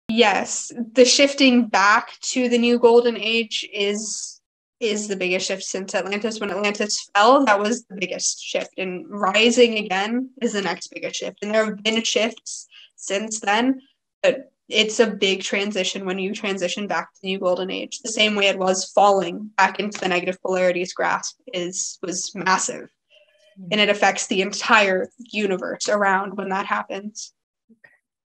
Q & A with the Elves | Is the Earth Shifting?
This is a clip from the Lions Gate event with the Elfian Realm.

For ages, the elves' stories have been miscommunicated, resulting in people forgetting the truth. The Elfian Realm has been working beyond the veil to help humanity reach the New Golden Age.

The Elfian Realm can step out from beyond the veil and communicate their true story in this direct channeling.

Podcast: An Hour in Golden Sunshine
Facebook: Kastalia Nova Theo
Facebook: The Realm of Inspiration